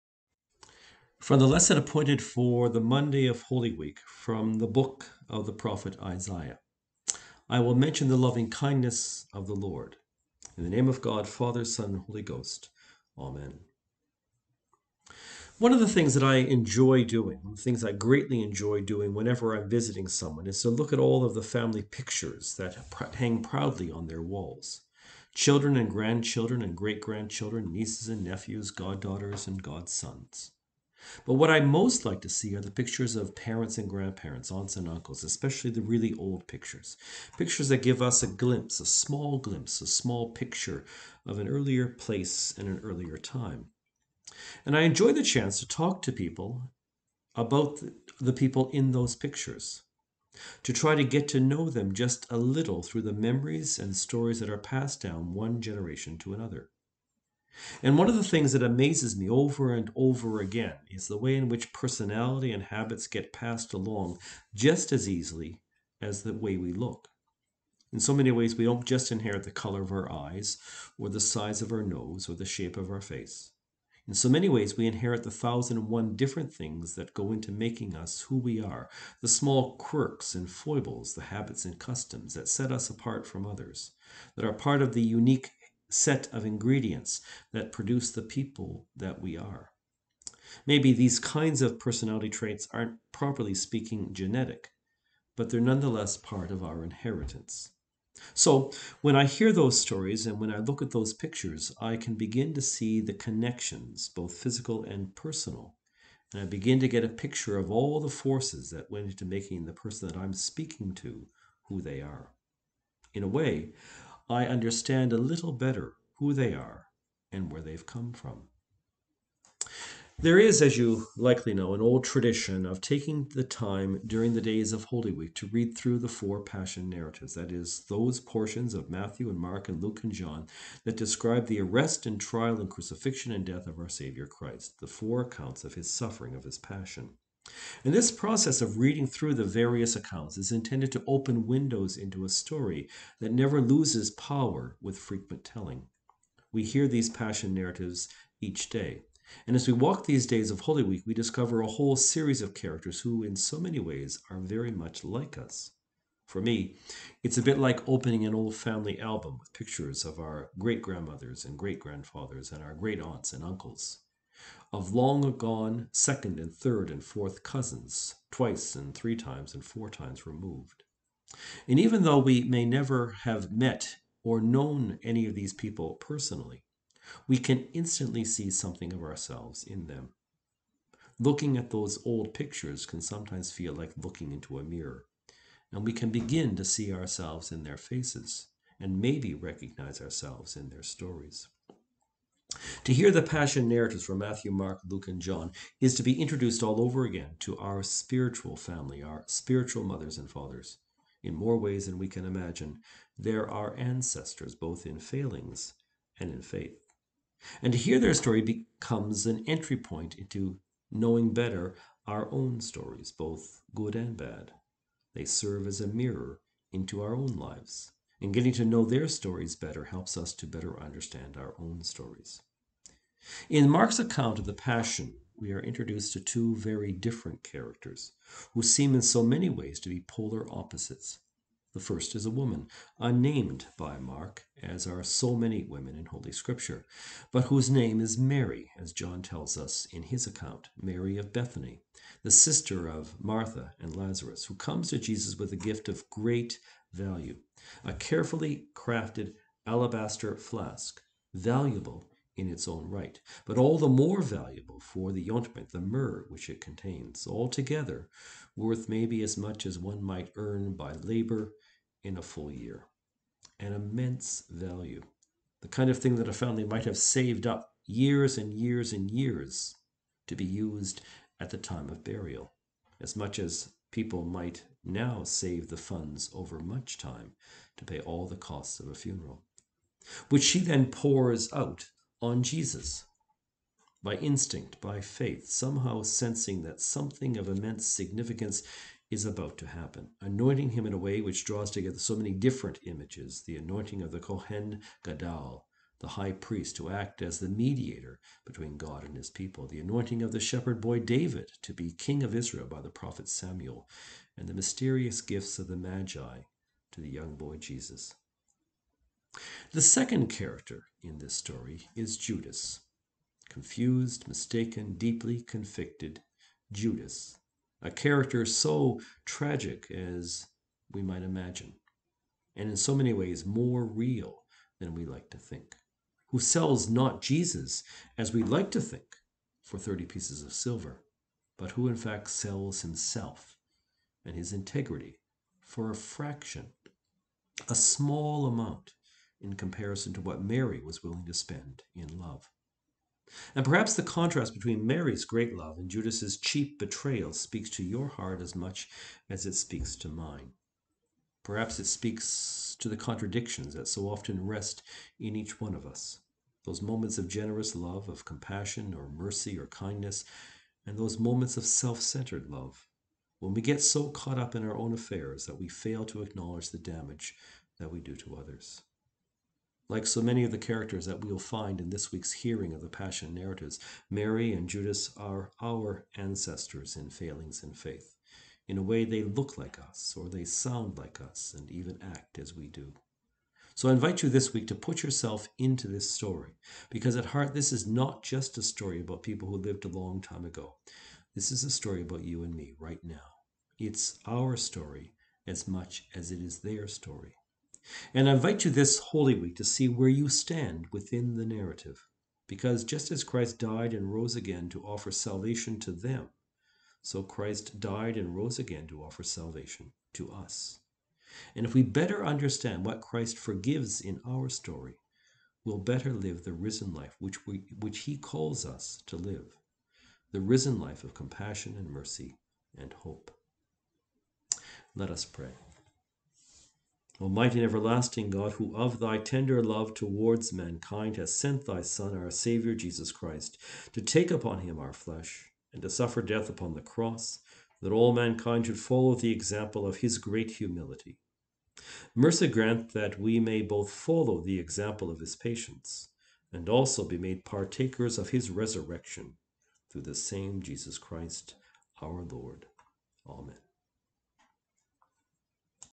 Meditation for the Monday in Holy Week 2023
Ancestors in the Faith: Mary and Judas